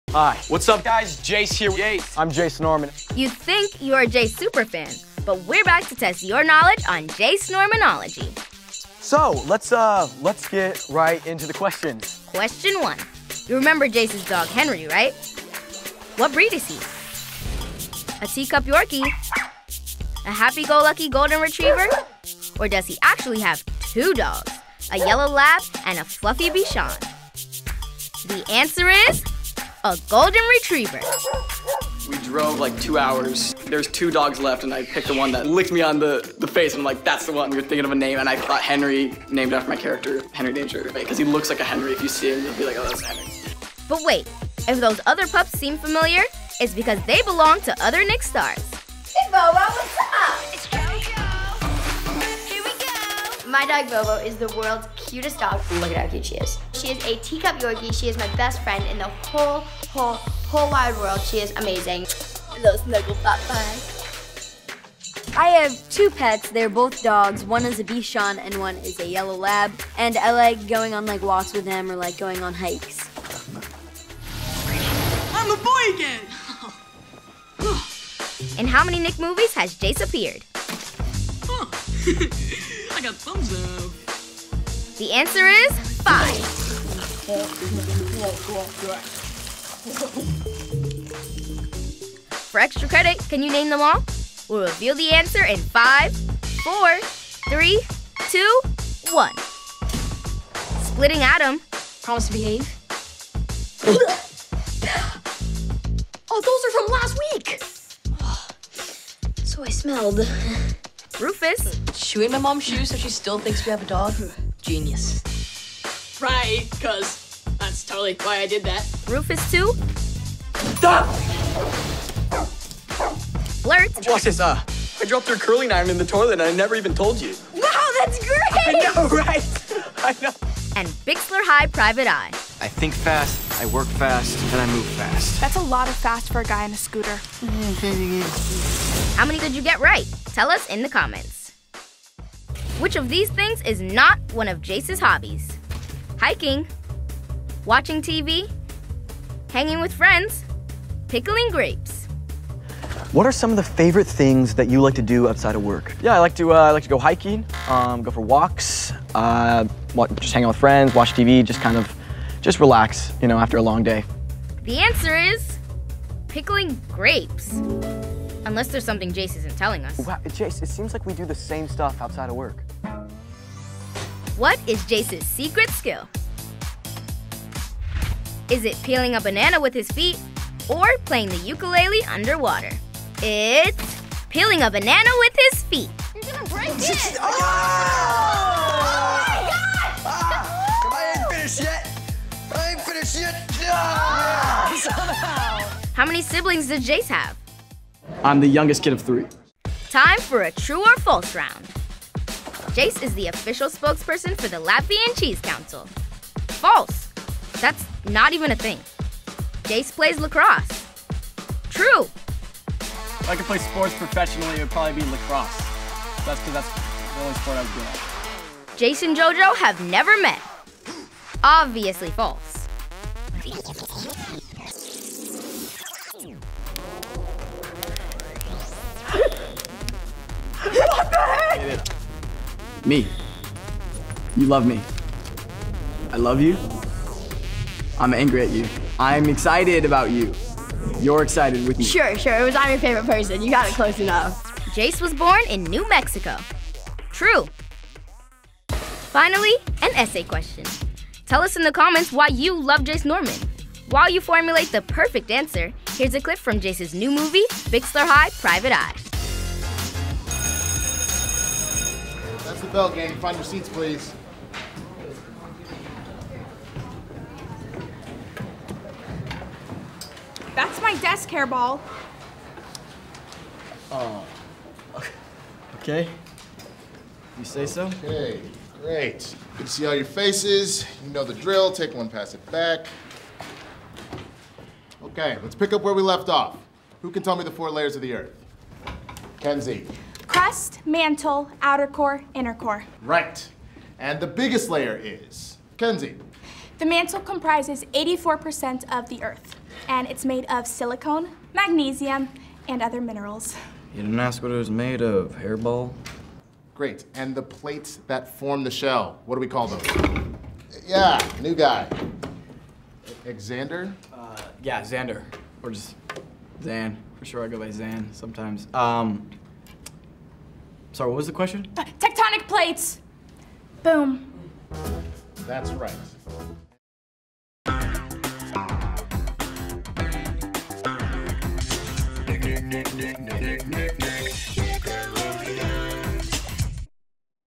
Can You Ace The Jace Norman Superfan Quiz?
Do you absolutely STAN Jace Norman? Prove your obsession with this Jace Norman quiz-- only **true** superfans will make it to the end 😜 You can catch Jace in the upcoming Nickelodeon original movie "Bixler High Private Eye," or fighting crime in Swellview on "Henry Danger." Looking for a little TBT Jace action? You can catch Jace's classic movies-- like "Blurt," "Rufus," and "Splitting Adam,"-- on TeenNick later this month!!!